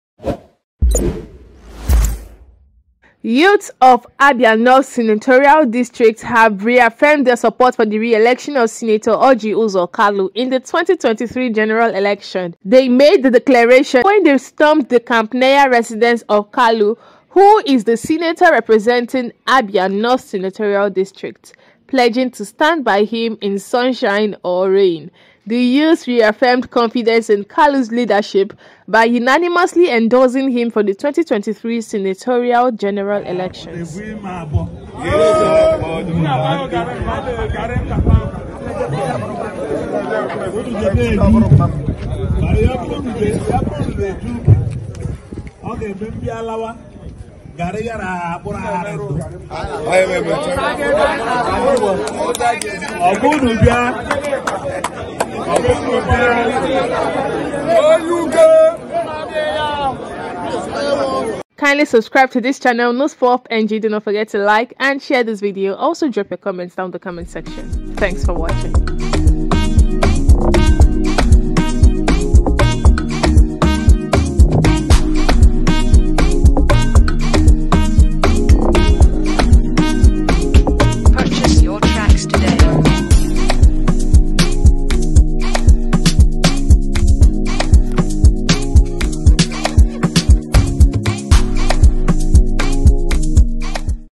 Video: Massive Crowd as Abia North Youth Storm Orji Uzor Kalu's Residence, Reaffirm Support For Him
Youths of the Abia North senatorial district pledge to support Orji Uzor Kalu in the forthcoming 2023 general elections. Watch to see how the youths assembled at Kalu's residence.